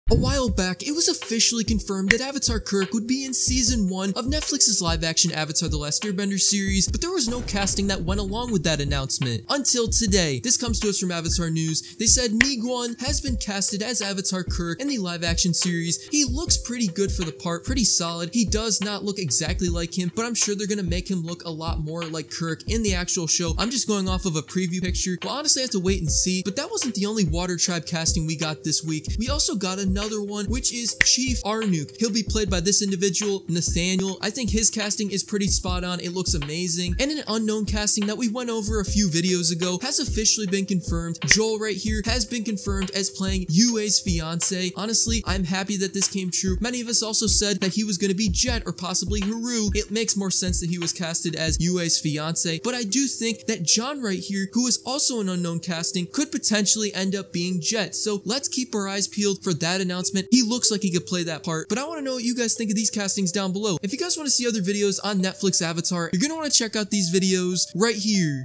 First Look at Avatar Kuruk - Netflix's Avatar Live Action
Netflix Avatar Live Action announced months ago that we would be seeing Avatar Kuruk in season 1, that casting has finally been revealed along many more castings.